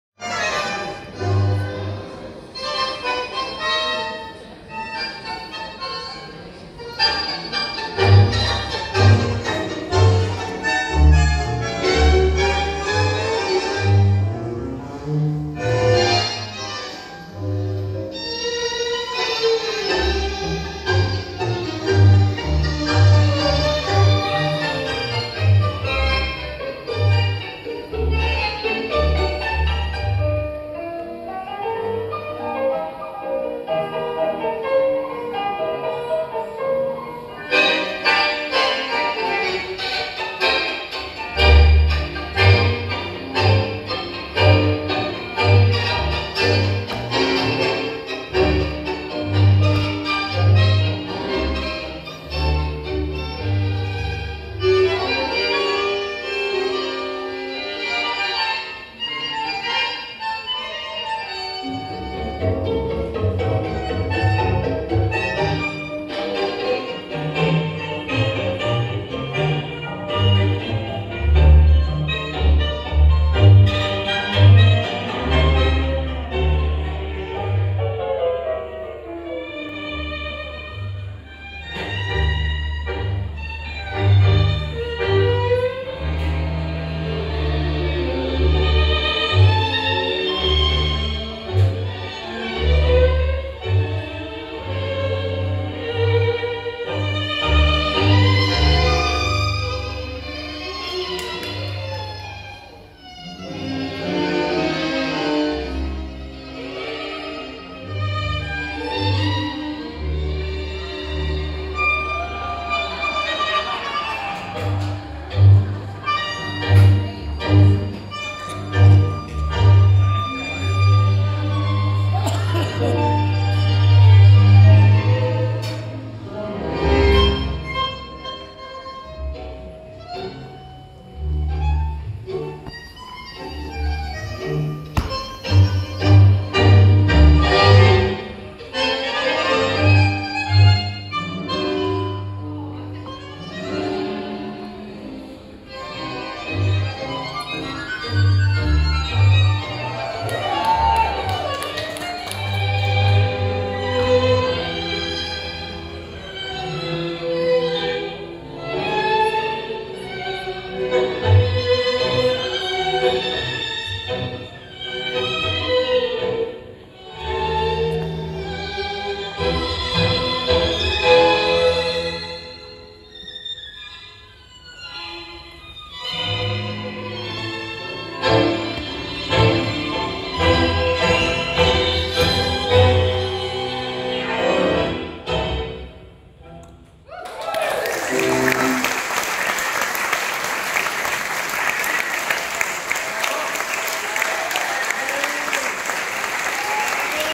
Ariadna Naveira and Fernando Sanchez perform at Milonga Volver in NYC - 3/4
Ariadna and Fernando perform at Milonga Volver by Sarah La Rocca and Jack Hanley